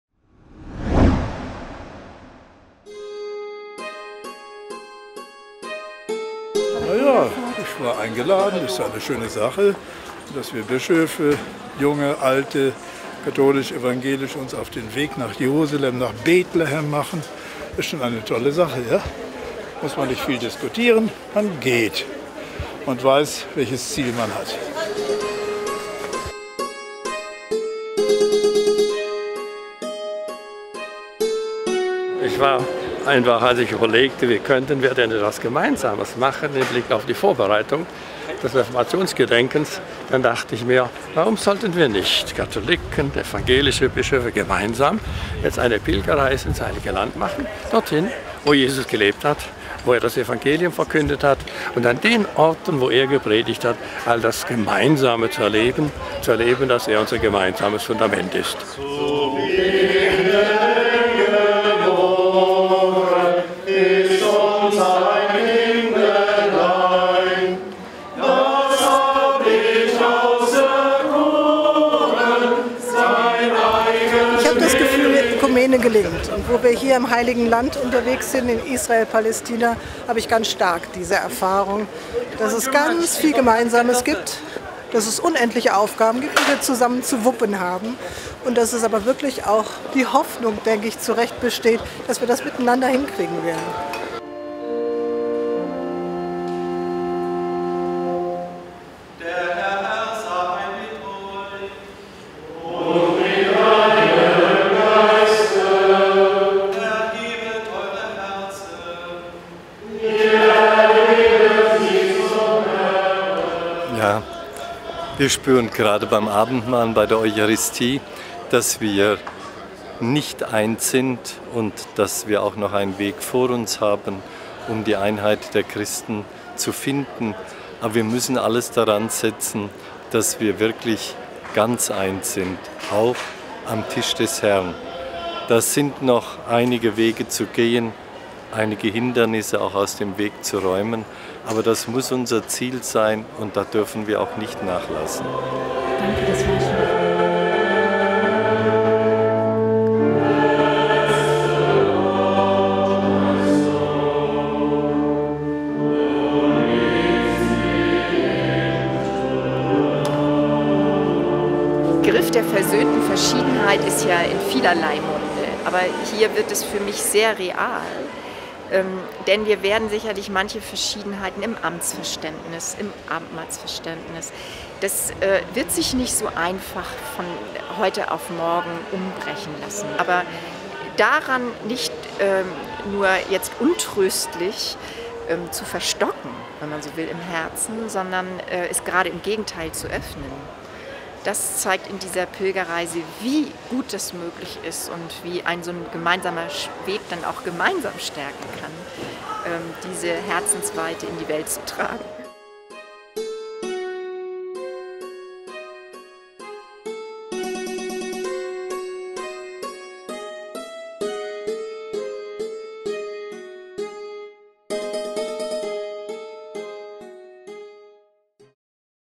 Ökumenische Pilgerreise: Auf dem Weg zum ganz großen Ziel
Evangelische und katholische Bischöfe pilgern erstmals gemeinsam durch das Heilige Land. Doch der Weg zu einer Einheit der Kirche scheint immer noch weit. Denn obwohl auf der Reise viel persönliche Sympathie und Nähe zu spüren sind, treten die trennenden Unterschiede der Konfessionen offensichtlich hervor.